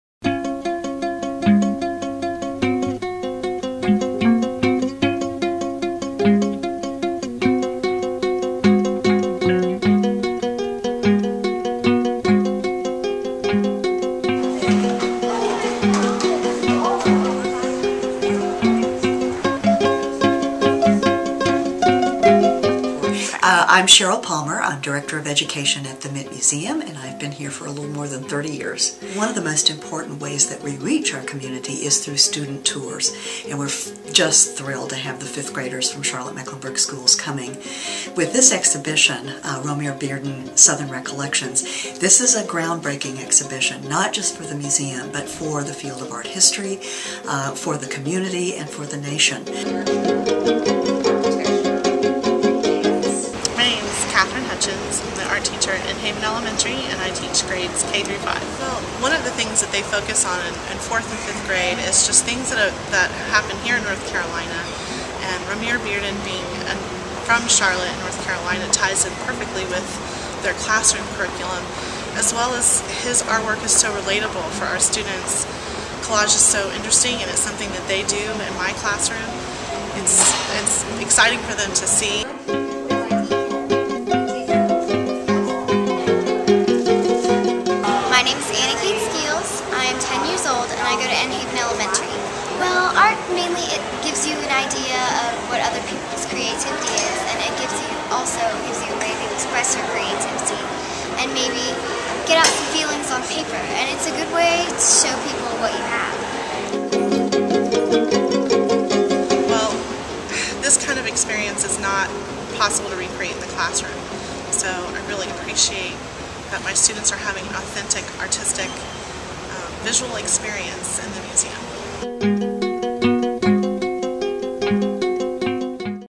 ASC Education: CMS Field Trip at the Mint Museum
For the 2011 Annual Fund Drive, the Arts & Science Council raised an additional $1 million to support arts, science and history programs in Charlotte-Mecklenburg Schools. Part of the investments ASC will make in education this year includes bringing 5th graders to the Mint Museum Uptown so they can experience Romare Bearden: Southern Recollections.